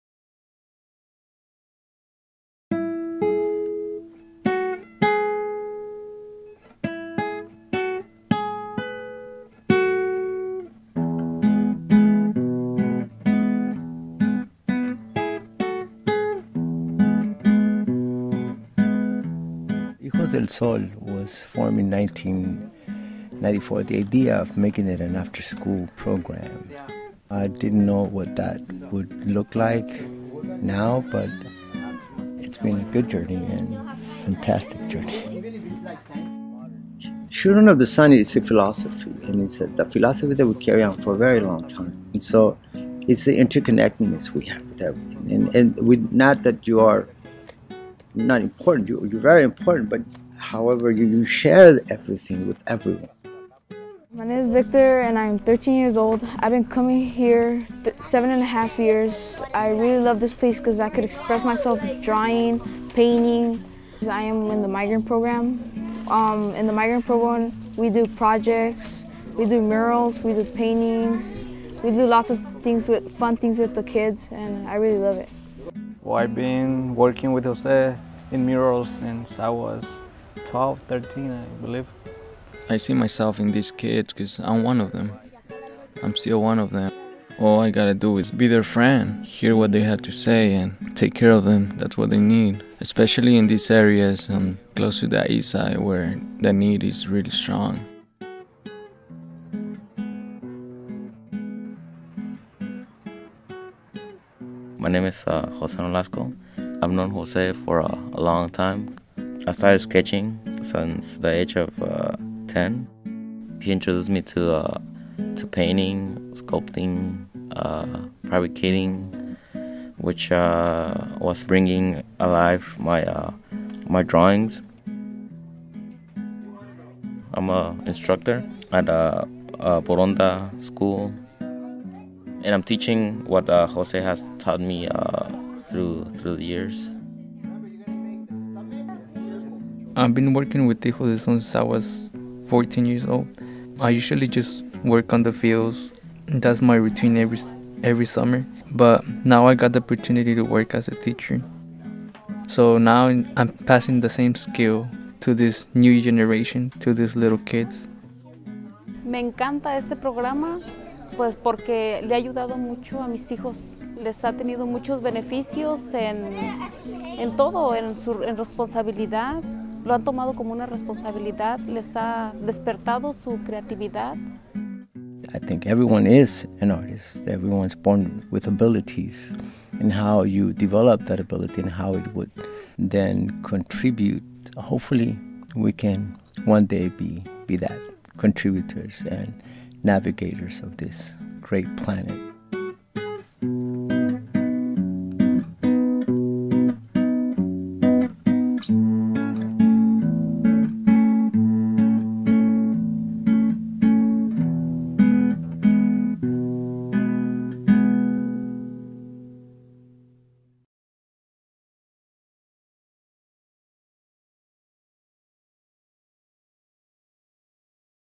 Hijos del Sol
Under the tutelage of muralist José Ortiz, the young illustrators and painters of Hijos del Sol (Children of the Sun) are teaching their skills to a new generation of artists in East Salinas. The Salinas Youth Initiative is created with support from The David & Lucile Packard Foundation.